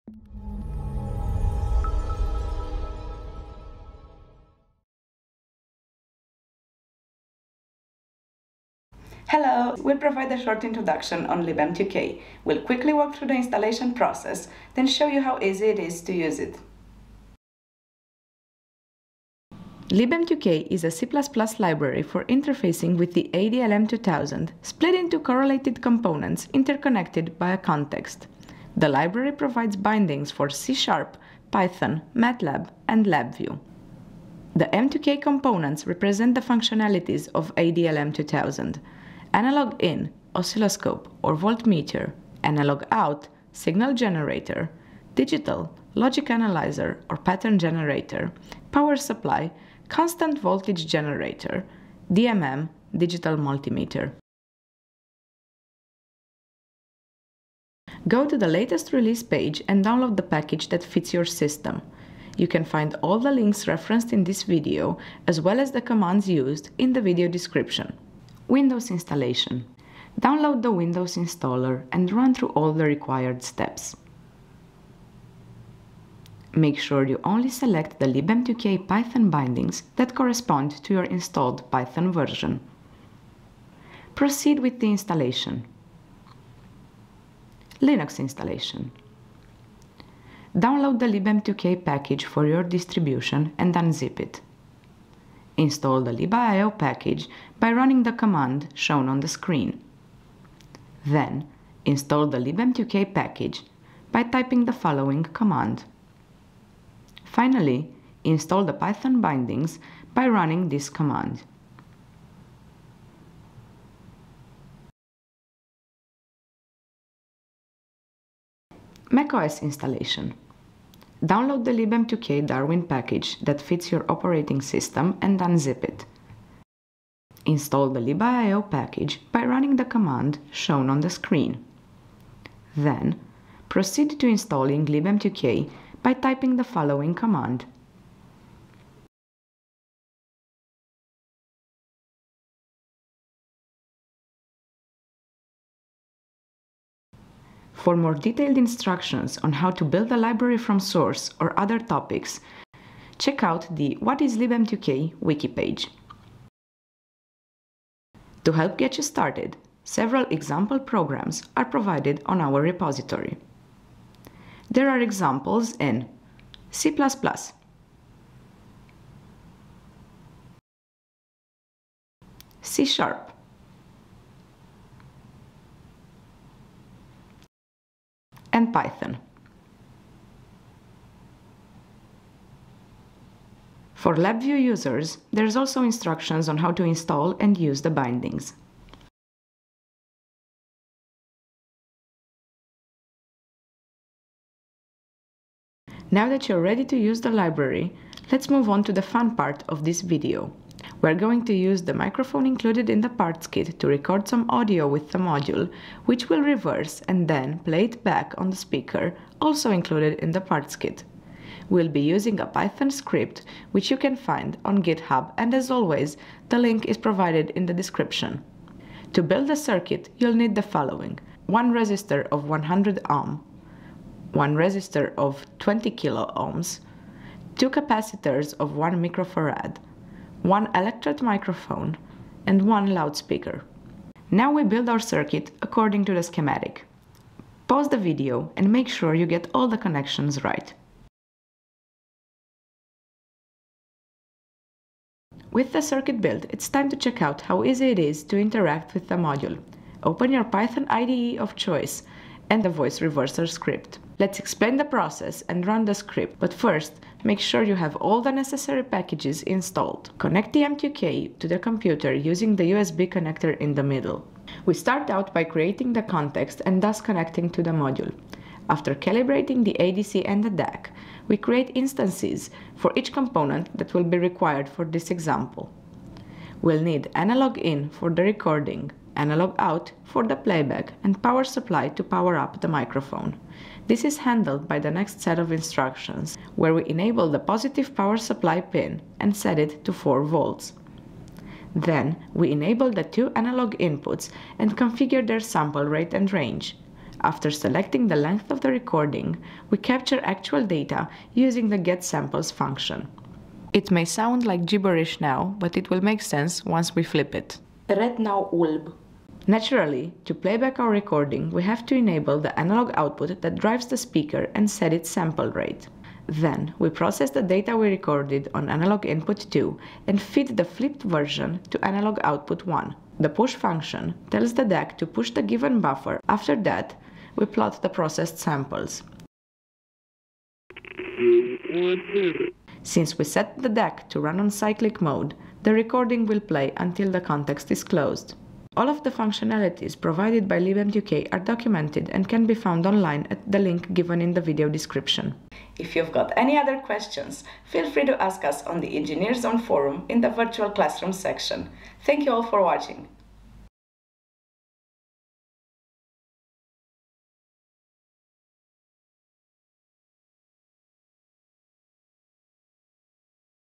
ADALM2000 Video Series: Video 13 - Introduction to libm2k
An instructional video on how to use the ADALM2000 hardware and create custom plugins and functions based on your application using an easy to use library.

This video walks through how to download and install the necessary software components on Windows, Linux, and Mac.  And also contains links to example programs written for all of the available bindings such as C++, C#, Python, and LabVIEW

Using a fun example project which utilizes the ADALM2000 and the ADALP2000, we'll walkthrough some of the key steps involved in using the library and getting started with it.

Commands
sudo apt install -f ./libiio-package.deb
sudo apt install -f ./libm2k-package.deb
sudo apt-get install ./python3-libm2k-package.deb.

Commands
sudo installer -pkg /path/to/libiio-package.pkg -target /
sudo installer -pkg /path/to/libm2k-package.pkg -target /